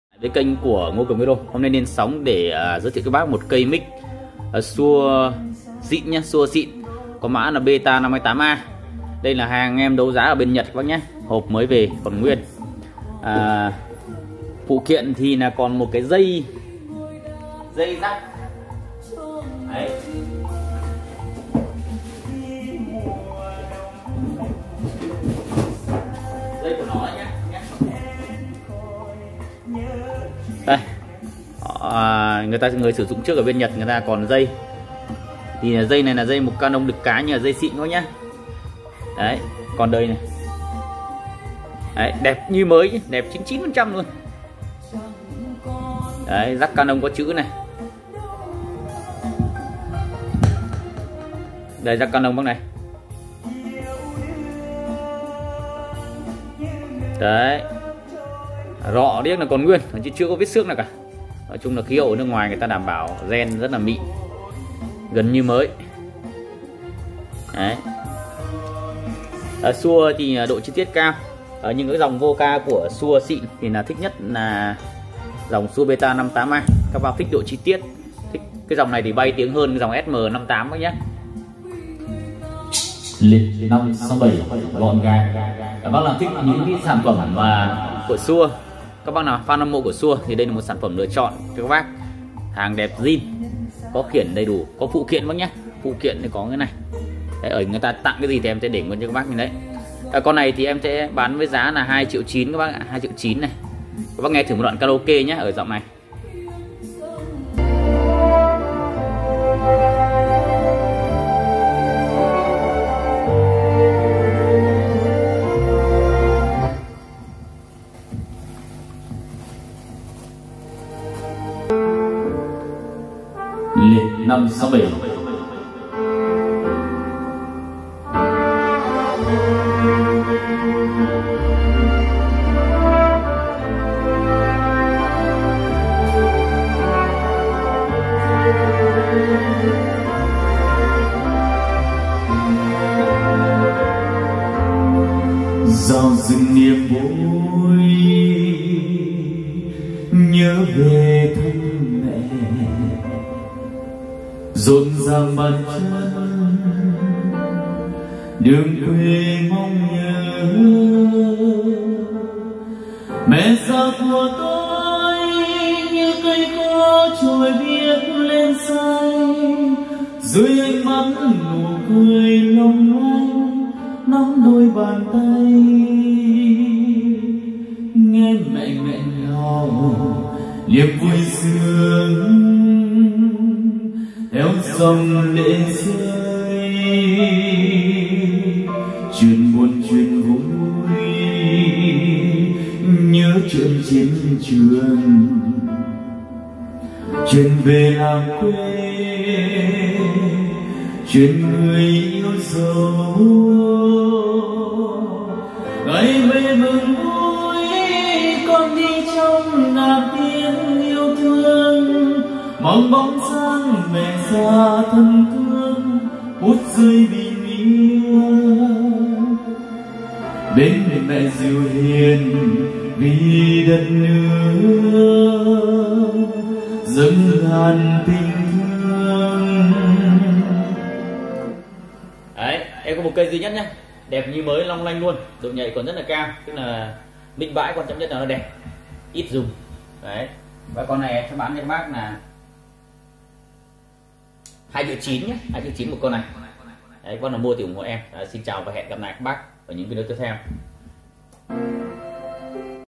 Shure beta 58a bãi hàng đấu giá Japan đẹp keng
Micro Shure Beta 58A được đánh giá có kích thước vô cùng nhỏ gọn, trọng lượng nhẹ nhàng chỉ 278g, tạo cảm giác vừa vặn, thoải mái cho người dùng khi cầm tay, hoạt động.

Với cấu tạo từ chất liệu hợp kim nhôm cứng cáp cùng tay cầm được phủ sơn nhám giúp chống bám vân tay và trầy xước khi có va chạm mạnh, đầu củ micro được bao bọc bằng lưới tản nhiệt cao cấp với các mắt đan xen nhau nhằm tăng cường hiệu quả tiếp nhận âm thanh và lọc âm.

Cùng với phần lưới tản nhiệt chắc chắn còn có tác dụng chống nổ, bảo vệ màng loa không bị hư hỏng trước những tác động mạnh từ bên ngoài. Tay cầm của được trang trí bằng tên logo của hãng cùng tên model của sản phẩm giúp người dùng dễ dàng nhận diện.